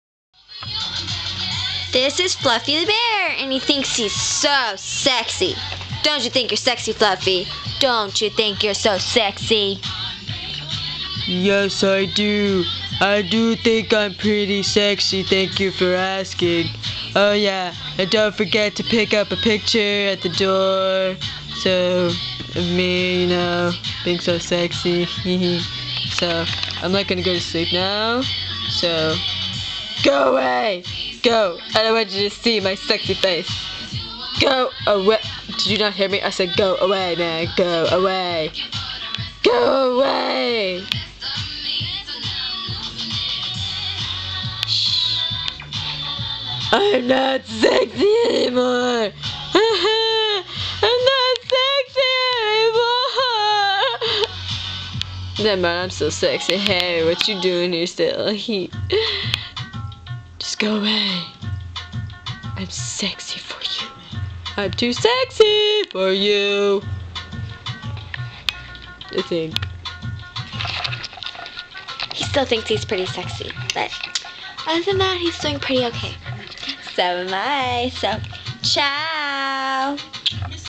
Sexy Bear
My daughter had no school and was dorking around...